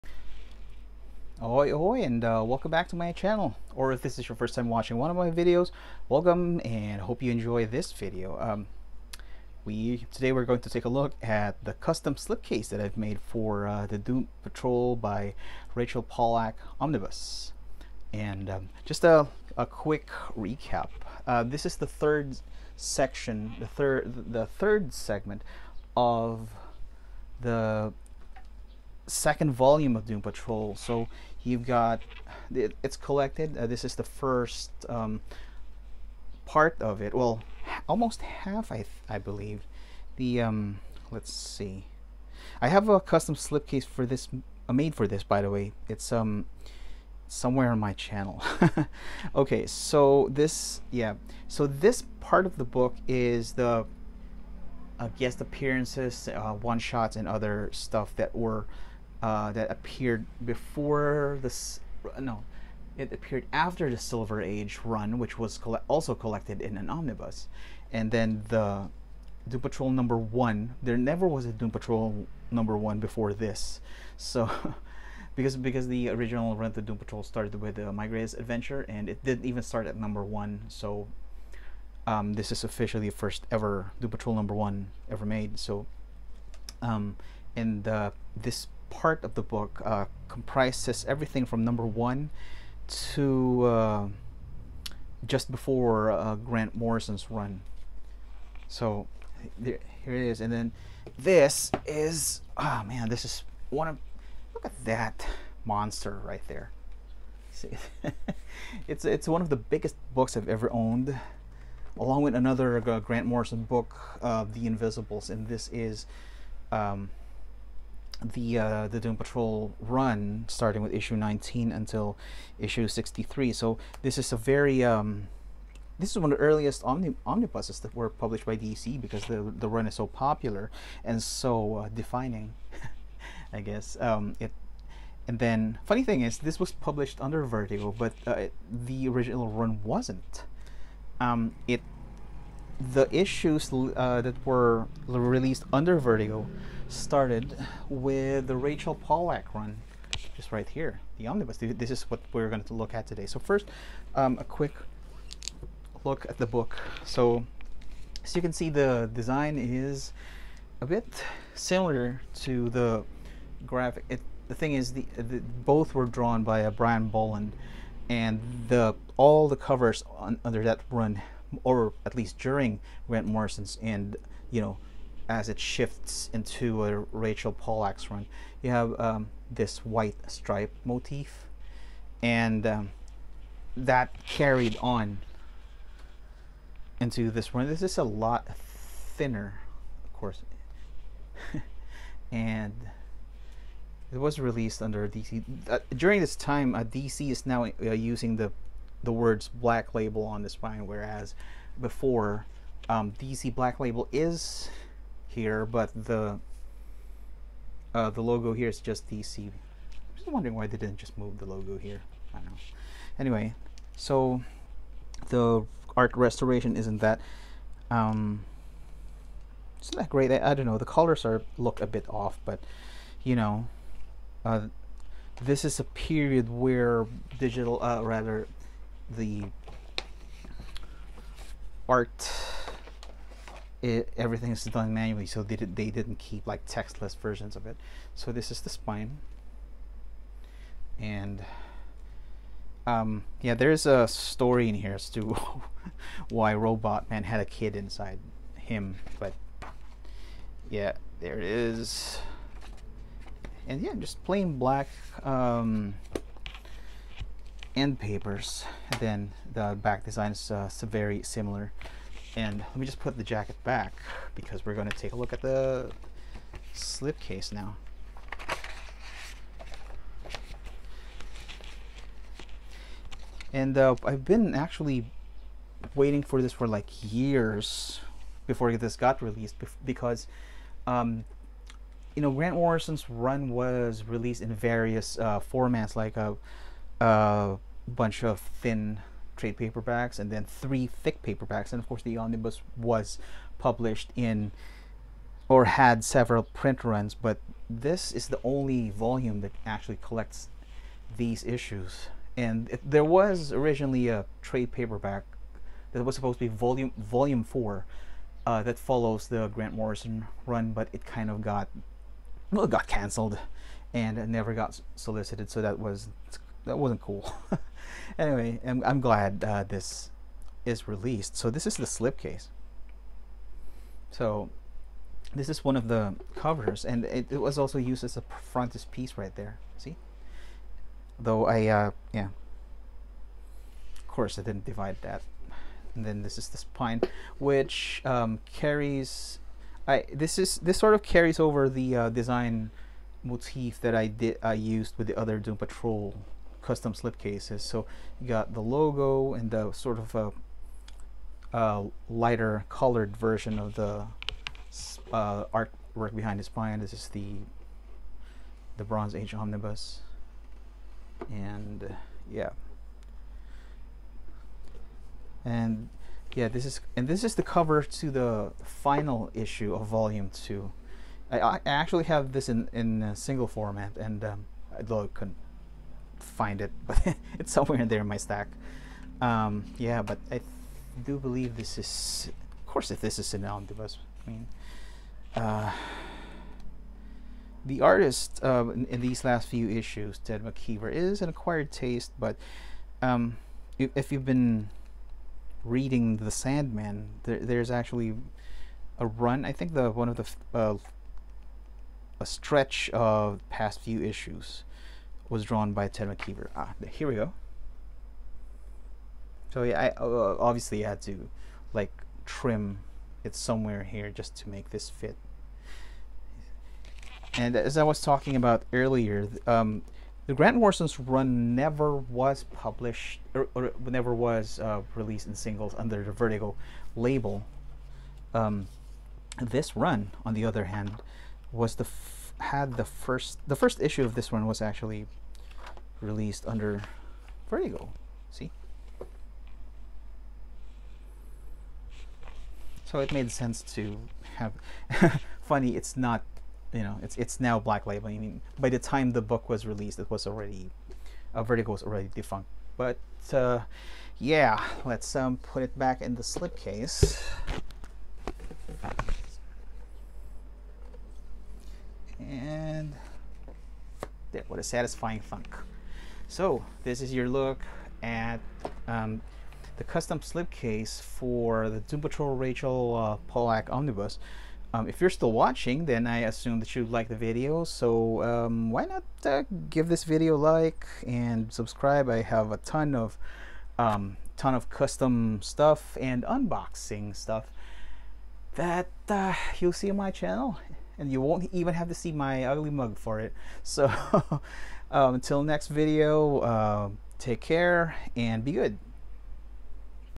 Custom Slipcase: Doom Patrol by Rachel Pollack Omnibus
Here's a look at the custom slipcase I built for the Doom Patrol by Rachel Pollack Omnibus, continuing Grant Morrison's acclaimed run and wrapping up Doom Patrol volume 2.

EDIT 5/29: I've just read Unstoppable Doom Patrol #3 and I just found out today that Rachel Pollack had passed away last month. We will remember you in the Dream Patrol.

Doom Patrol and all related characters are © and published by DC Comics.